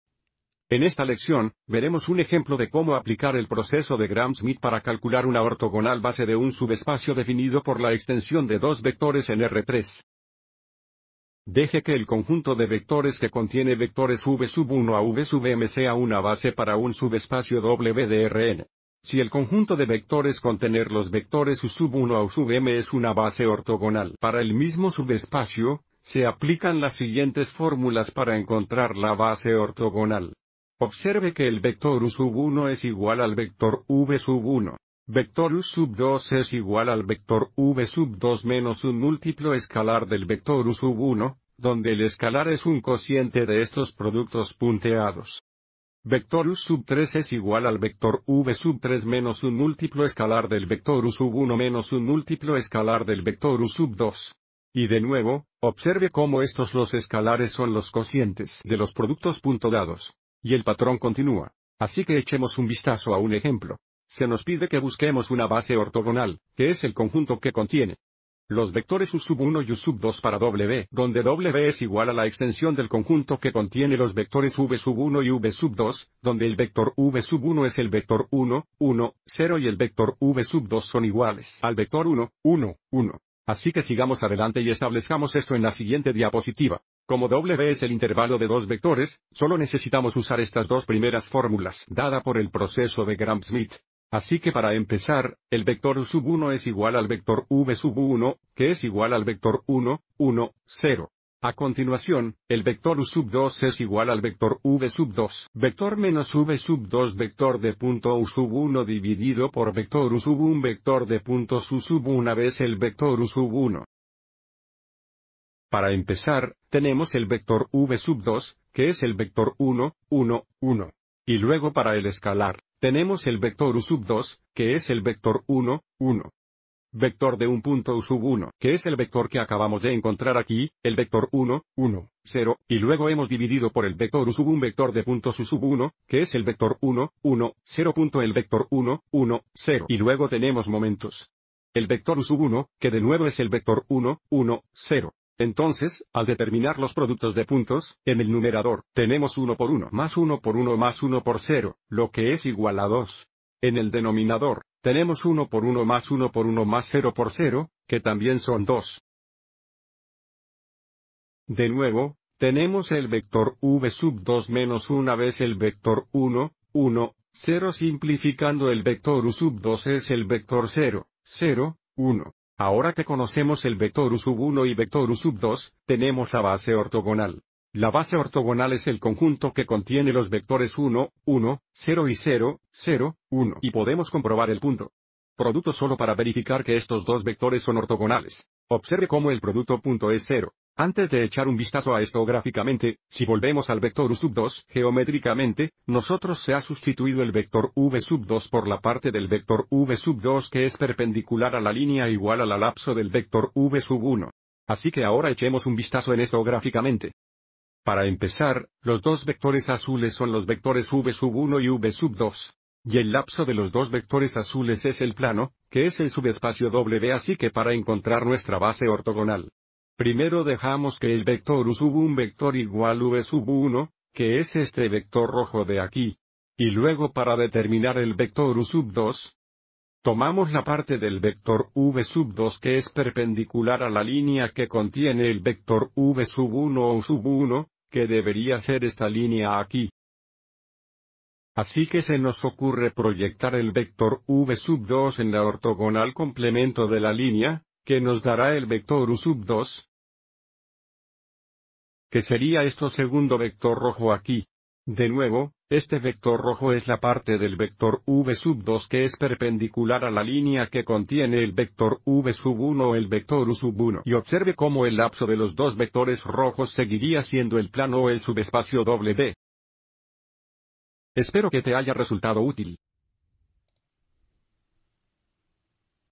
Proceso de Gram Schmidt: encontrar una base ortogonal (2 vectores en R3)
Este vídeo explica cómo determinar una base ortogonal dada la base de un subespacio.